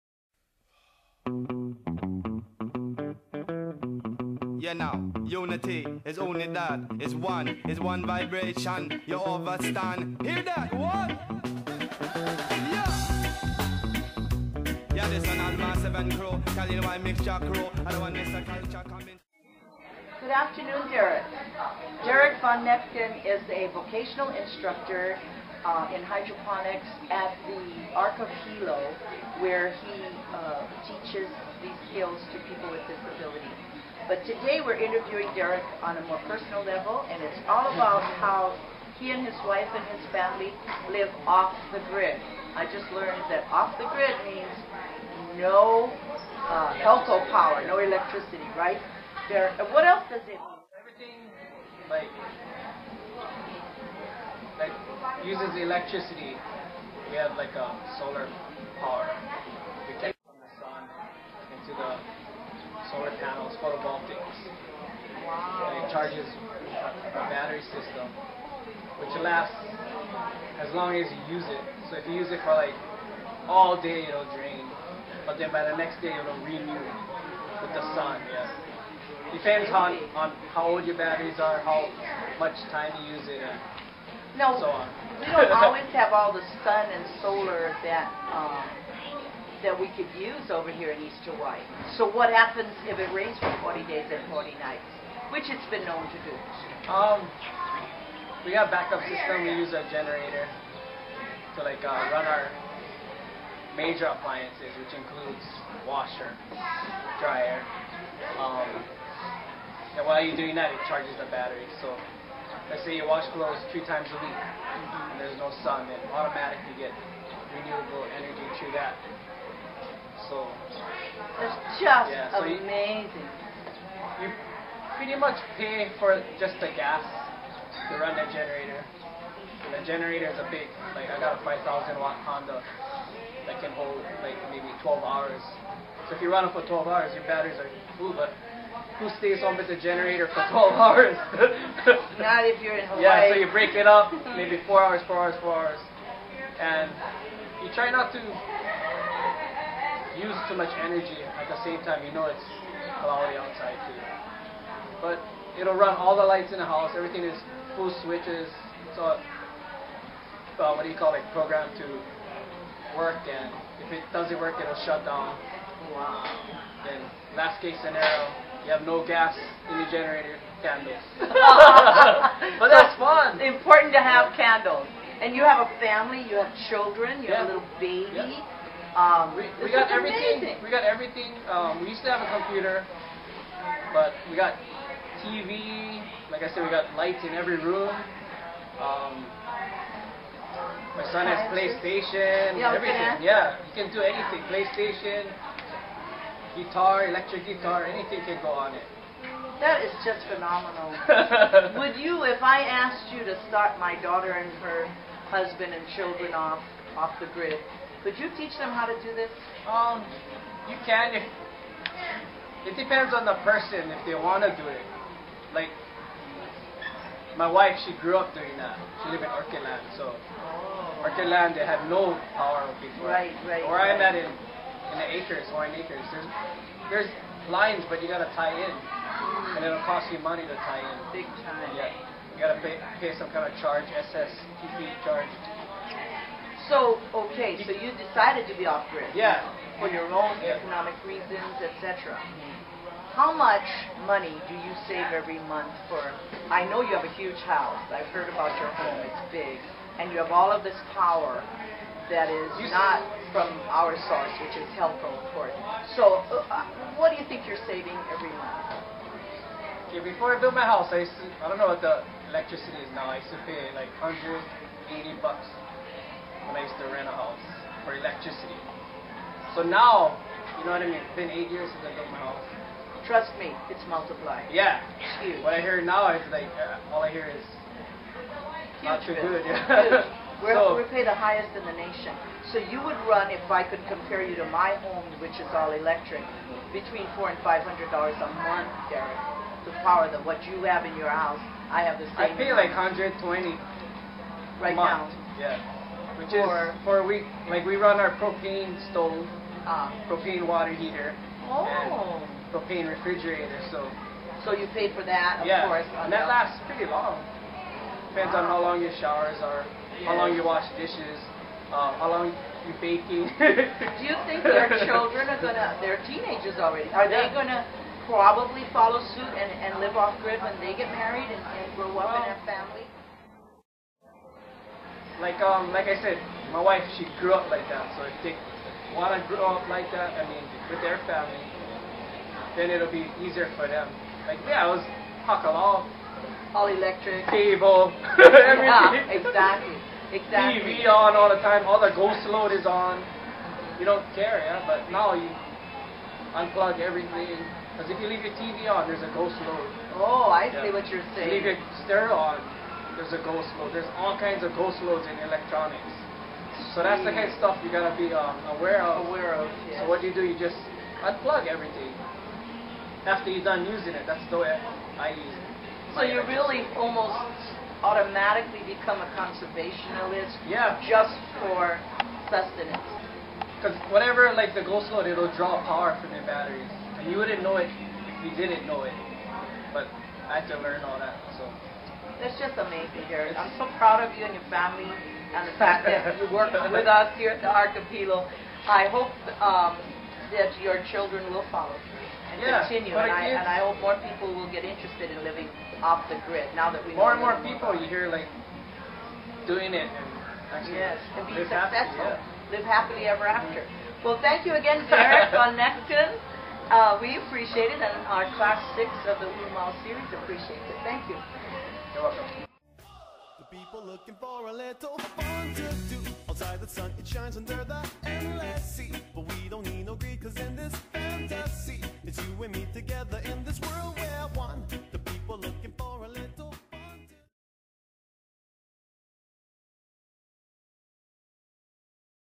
Derek Von Nechten, Living Off-the-Grid
A conversational interview with Derek Von Nechten at the Arc of Hilo with Charmaine.  He describes his way of life of energy-independence, and includes renewable sources such as photovoltaic, alternative power generation (gas generator), and water-catchment.